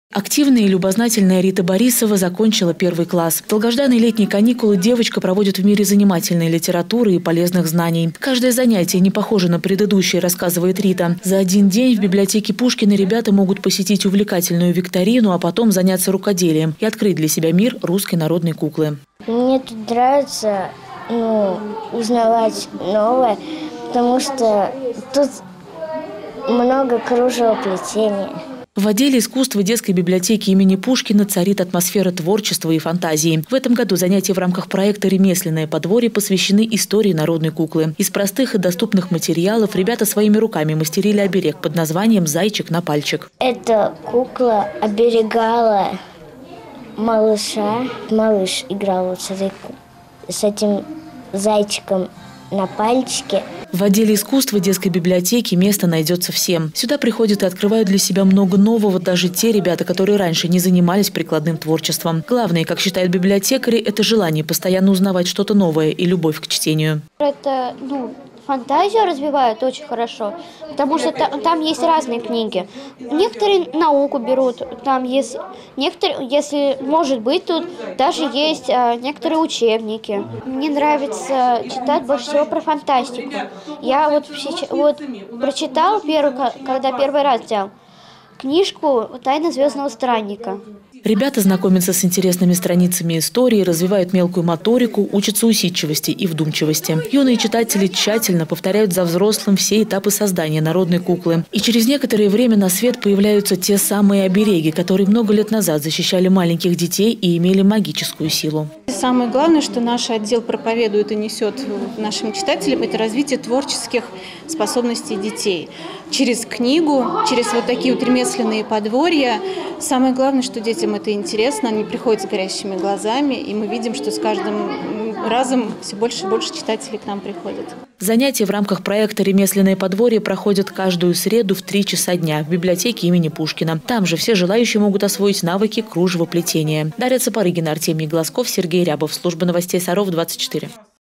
Ремесленное подворье
В детской библиотеке имени Пушкина прошло очередное занятие в рамках проекта «Ремесленное подворье». Ребята познакомились с историей традиционной народной куклы и своими руками попробовали смастерить оберег.